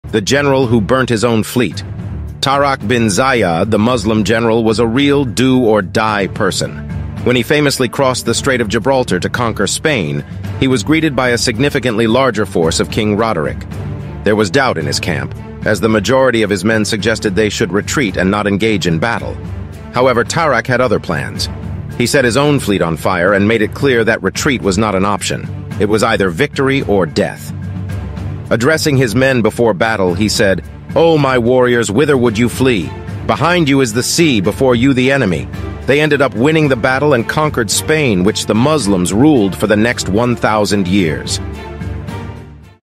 The general Burning His Fleet!
Tarak bin Zayyad was a real do-or-die leader! When his forces arrived in Spain to battle King Roderick's much larger army, he made a brave decision: burn his own fleet so that his men had no way to retreat and had to stay and fight!